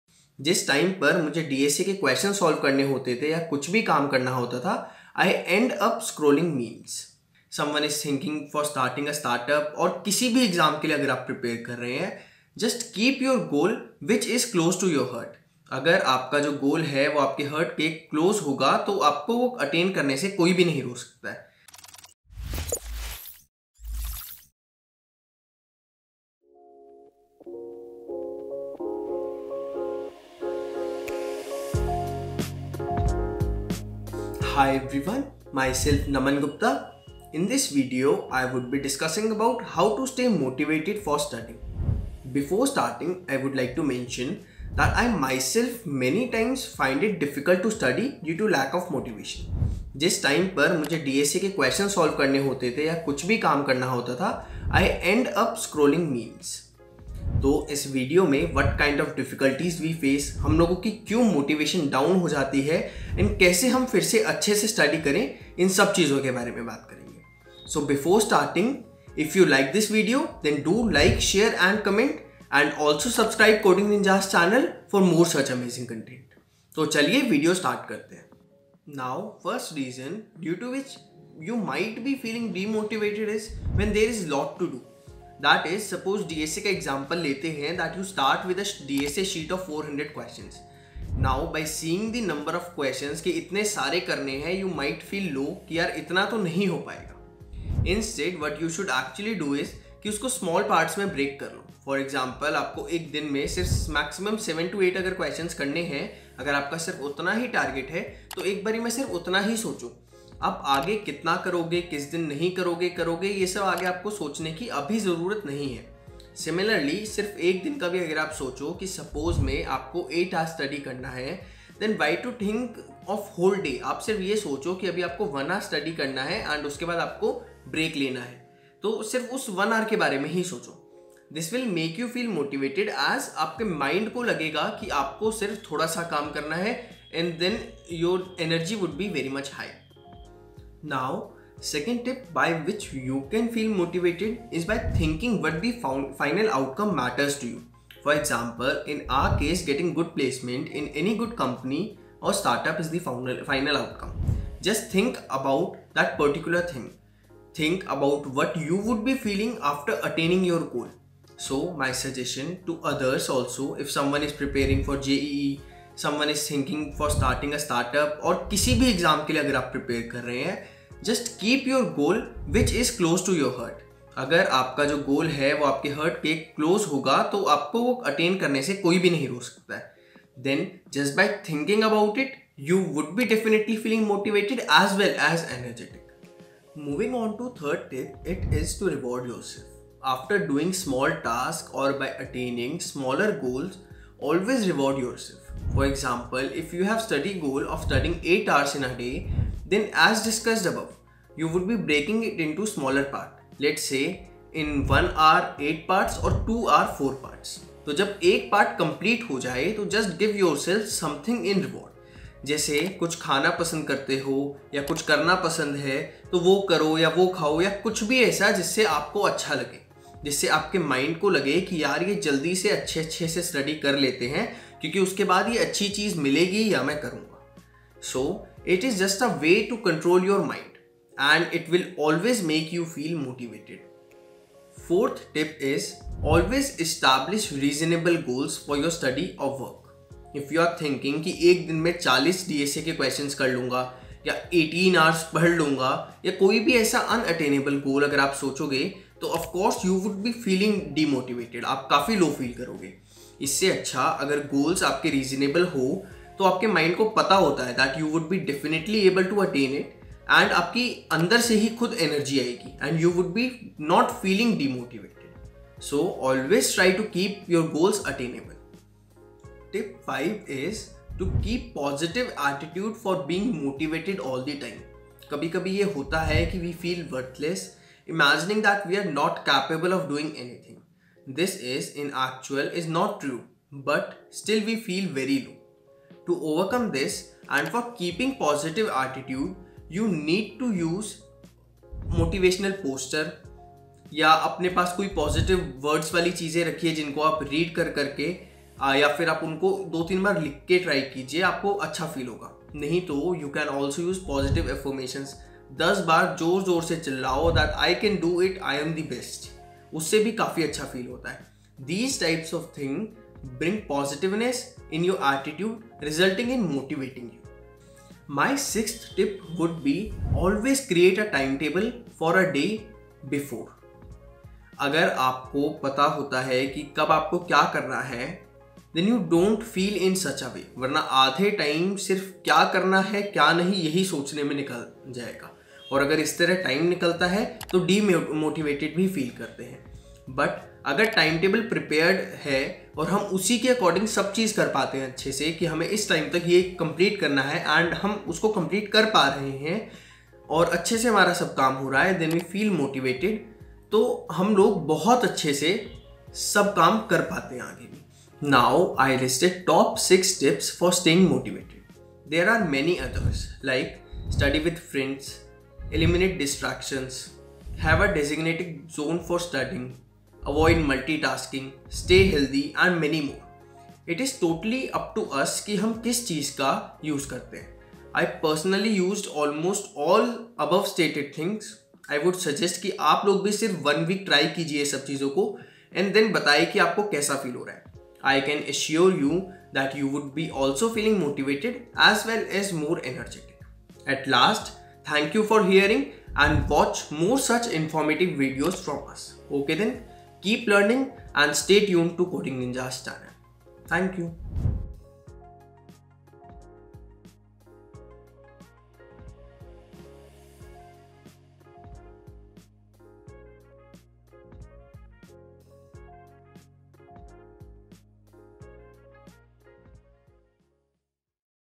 How To Stay Motivated For Studying?| How to concentrate on studies | Study Motivation |Coding Ninjas
BEST FREE Resource To Learn DSA, C++, Python, Java, Web Development, Competitive Programming,

This video will discuss how you can stay motivated by changing a few things in life, like not trying to do multiple tasks simultaneously, always trying to motivate yourself by setting goals, and, if possible, preparing a timetable that will help you stay positive.

CHAPTERS

0:34 - Introduction
1:18 - Too Many Tasks
2:20 - Think about the Final Outcome
3:06 - Reward Yourself
3:54 - Establish Reasonable Goals
4:29 - Positive Attitude
5:18 - Create a Timetable
6:01 - Additional Tips

How To Stay Motivated For Studying?